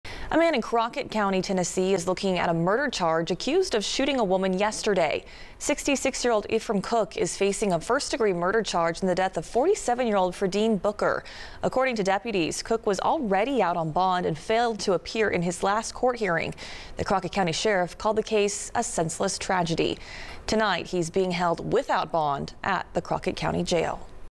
Gadsden man charged with murdering woman
A man has been charged with first-degree murder after Crockett County deputies say he fatally shot a woman on Saturday.